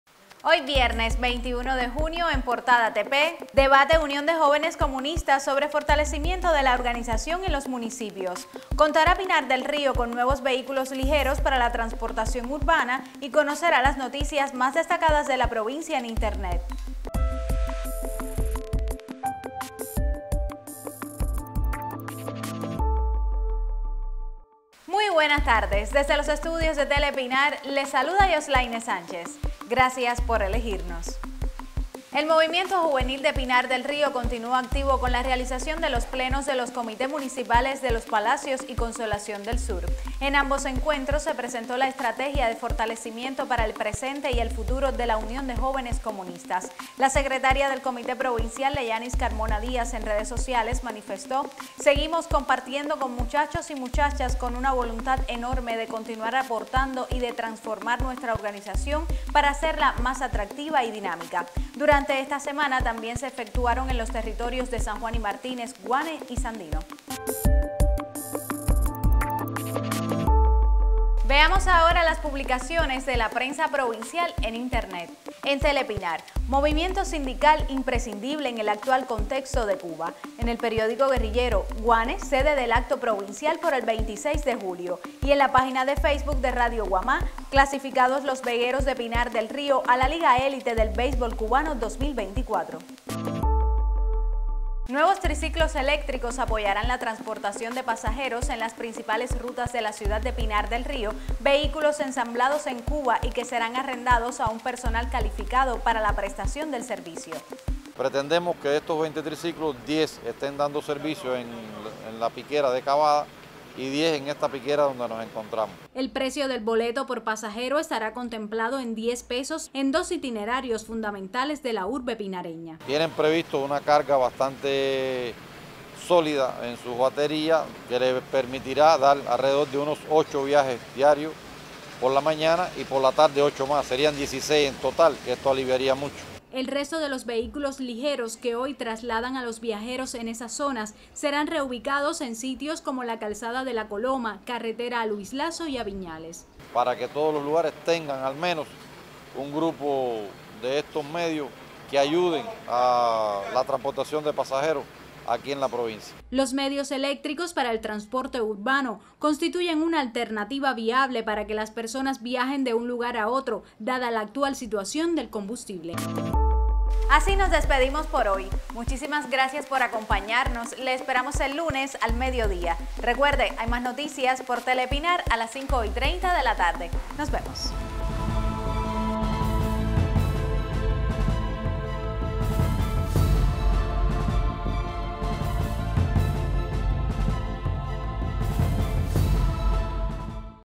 Las noticias de Pinar del Río al mediodía.
Temas:
✅Debate UJC sobre fortalecimiento de la organización y los municipios.
✅Contará Pinar del Río con nuevos vehículos ligeros para la transportación urbana.
✅Como es habitual conocerá las principales noticias que comparte hoy en portada web la prensa pinareña.